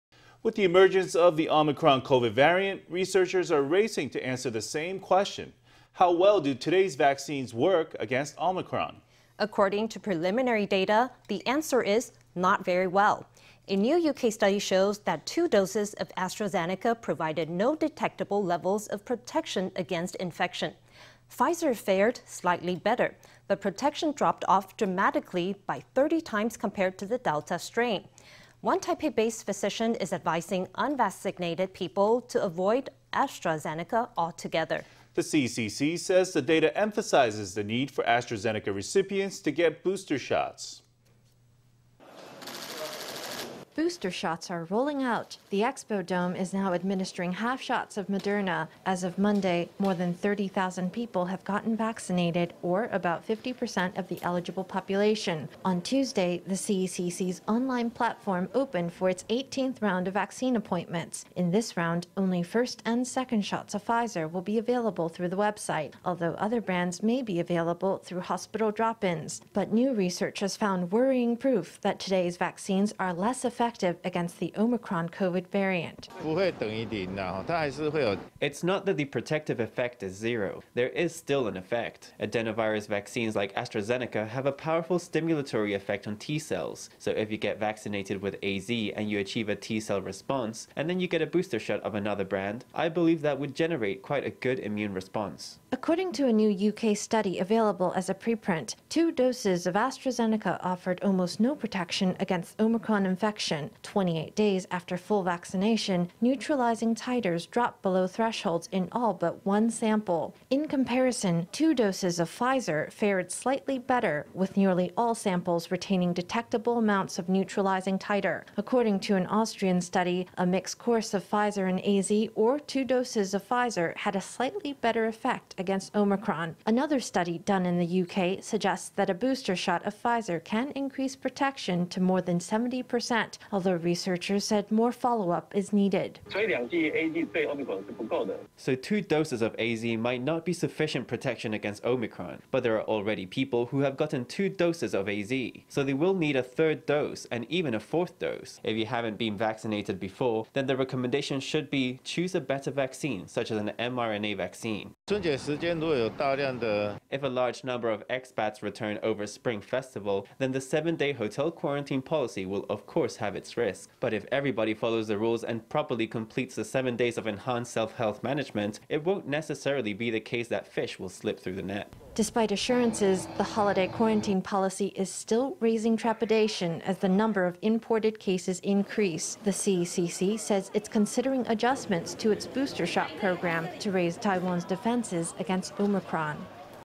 Doctor advises against AZ after concerning omicron studies
With the emergence of the omicron COVID variant, researchers are racing to answer the same question: How well do today’s vaccines work against omicron? According to preliminary data, the answer is: Not very well. A new U.K. study shows that two doses of AstraZeneca provided no detectable levels of protection against infection. Pfizer fared slightly better, but protection dropped off dramatically by 30 times compared to the delta strain. One Taipei-based physician is advising unvaccinated people to avoid AstraZeneca altogether. The CECC says the data emphasizes the need for AstraZeneca recipients to get booster shots.

Booster shots are rolling out. The Expo Dome is now administering half shots of Moderna. As of Monday, more than 30,000 people have gotten vaccinated, or about 50% of the eligible population. On Tuesday, the CECC’s online platform opened for its 18th round of vaccine appointments. In this round, only first and second shots of Pfizer will be available through the website, although other brands may be available through hospital drop-ins. But new research has found worrying proof that today’s vaccines are less effective against the omicron COVID variant.

Lee Ping-ing
CECC specialist advisory panel
It’s not that the protective effect is zero. There is still an effect. Adenovirus vaccines like AstraZeneca have a powerful stimulatory effect on T cells, so if you get vaccinated with AZ and you achieve a T cell response, and then you get a booster shot of another brand – I believe that would generate quite a good immune response.

According to a new U.K. study available as a preprint, two doses of AstraZeneca offered almost no protection against omicron infection. Twenty-eight days after full vaccination, neutralizing titers dropped below thresholds in all but one sample. In comparison, two doses of Pfizer fared slightly better, with nearly all samples retaining detectable amounts of neutralizing titer. According to an Austrian study, a mixed course of Pfizer and AZ or two doses of Pfizer had a slightly better effect against omicron. Another study done in the U.K. suggests that a booster shot of Pfizer can increase protection to more than 70%, although researchers said more follow-up is needed.

Huang Li-min
NTU Children’s Hospital head
So two doses of AZ may not be sufficient protection against omicron, but there are already people who have gotten two doses of AZ. So they will need a third dose and even a fourth dose. If you haven’t been vaccinated before, then the recommendation should be, choose a better vaccine, such as an mRNA vaccine.

Lee Ping-ing
If a large number of expats return over Spring Festival, then the seven-day hotel quarantine policy will of course have its risks. But if everybody follows the rules and properly complete the seven days of enhanced self-health management, it won’t necessarily be the case that fish will slip through the net.

Despite assurances, the holiday quarantine policy is still raising trepidation as the number of imported cases increase. The CECC says it’s considering adjustments to its booster shot program, to raise Taiwan’s defenses against omicron.